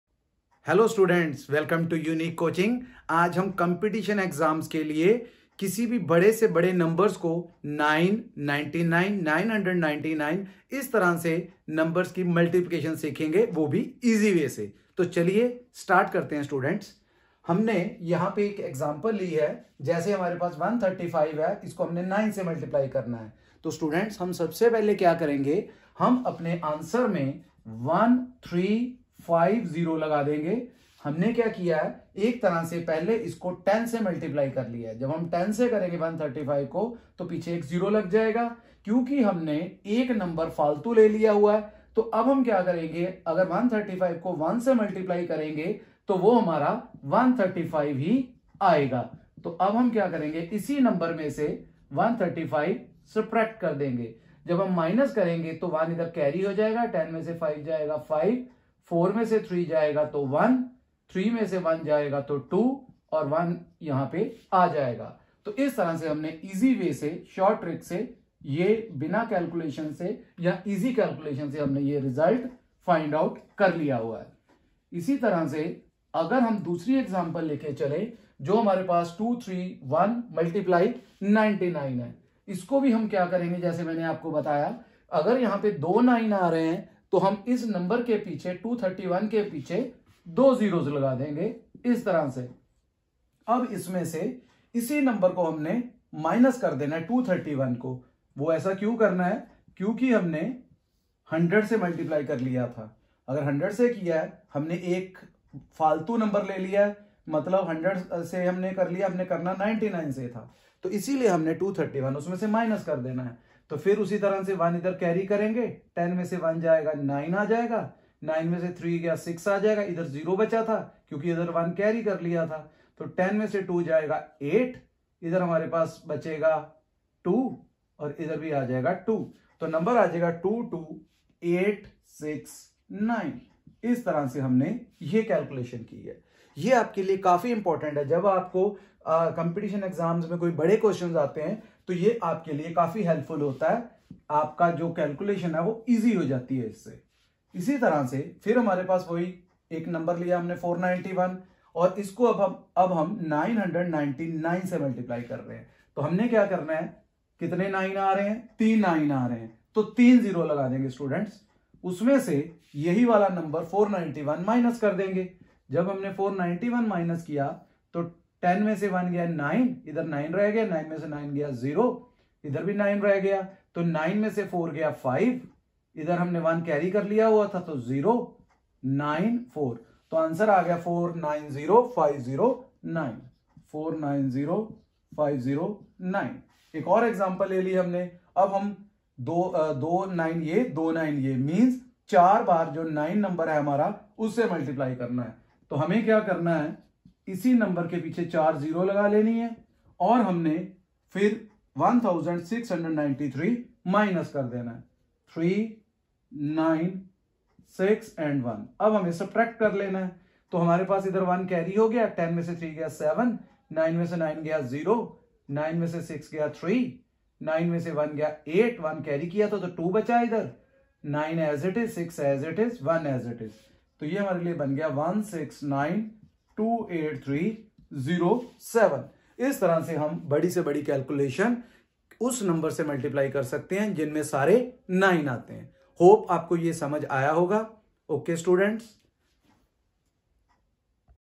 trick multiplication of any number by 9,99,999,9999….🔥🔥| unique coaching.
competitive exams concept

Hello Viewers
 Welcome to Unique Coaching

Iss video mein maine aap ko bahut hee short trick se multiplication concept samjhaya hai.
Iss video mein kisi bhi number ko 9,99,999etc se multiplication karna shikhaya hai.
Yeh concept aap ko competitive and government exams mein helpful ho ga.